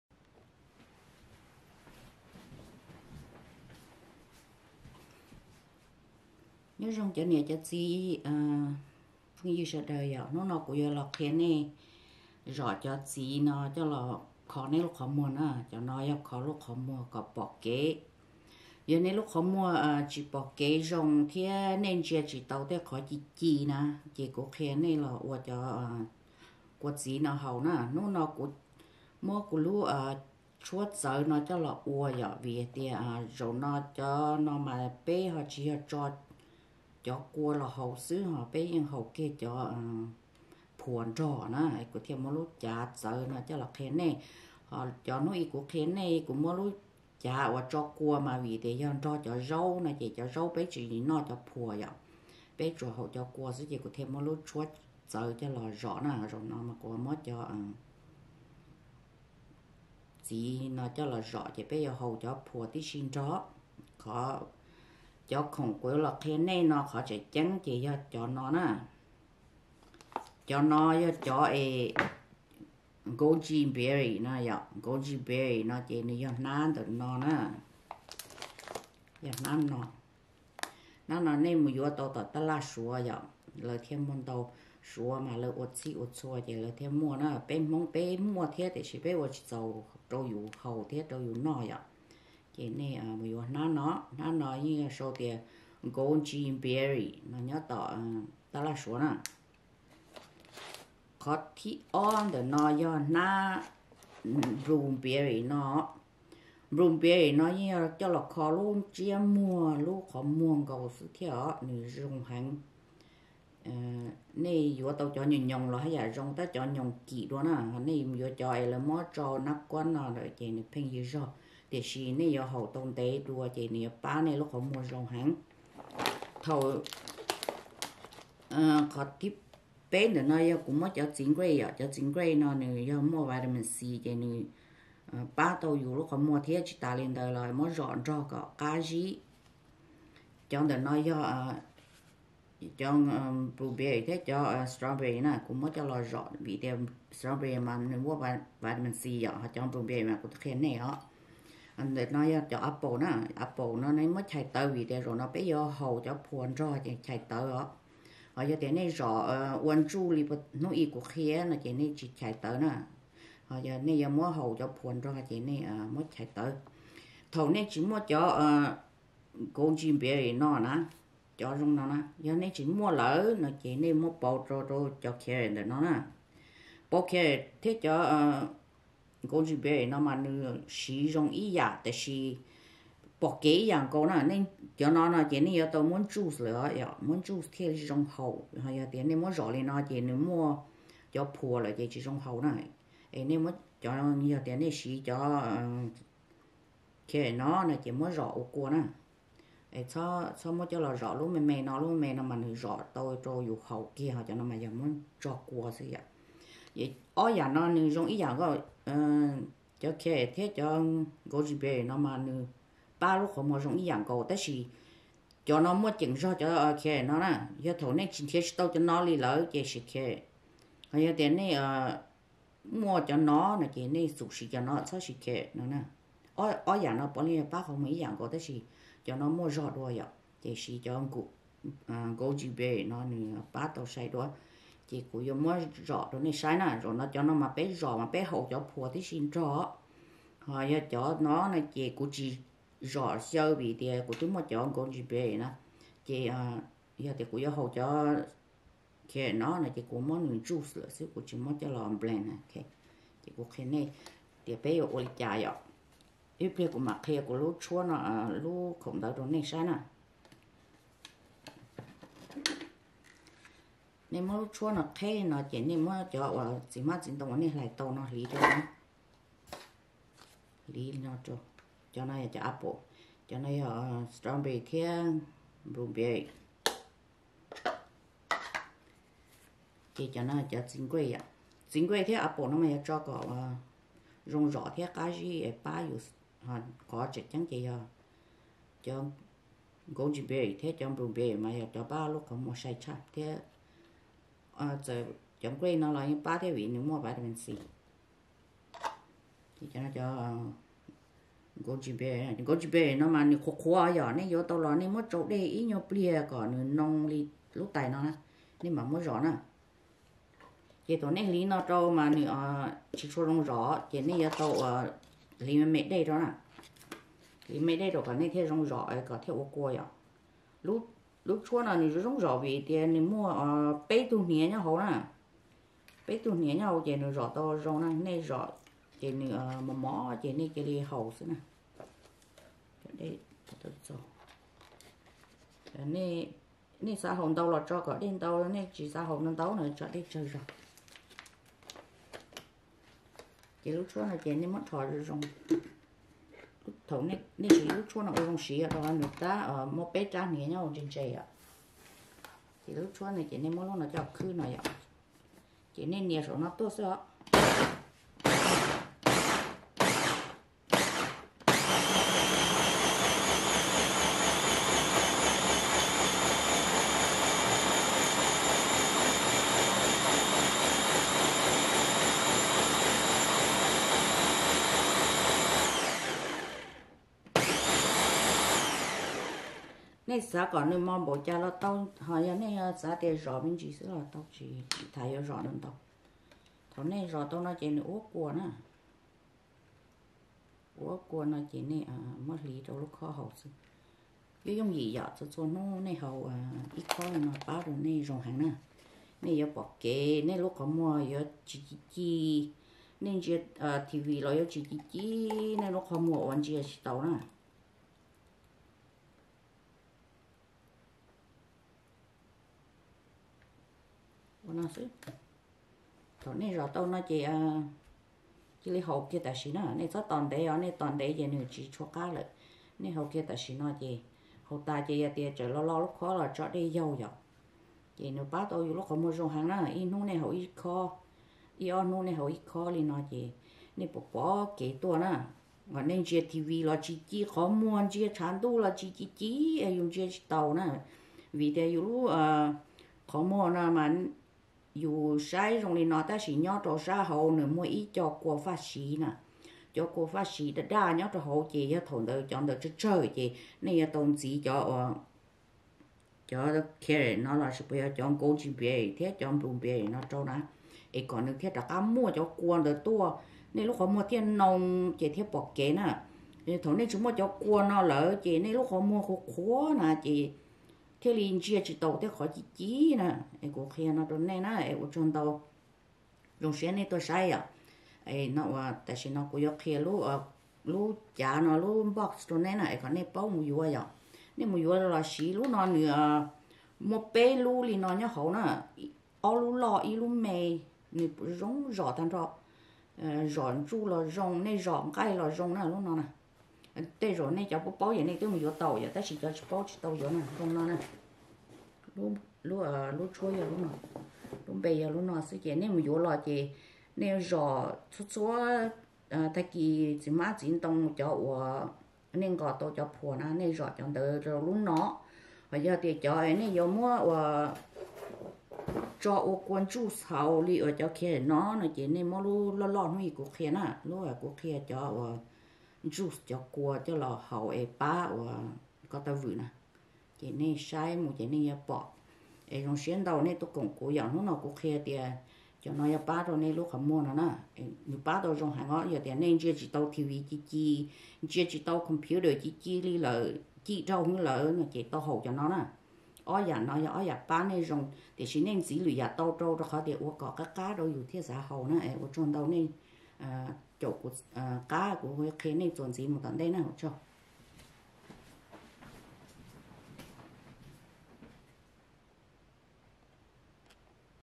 Tshuaj qhov muag pom kev/eye health
Home made juice with blends of goji berries, blueberries, grapes, strawberries, and apple for eyesight and vision health.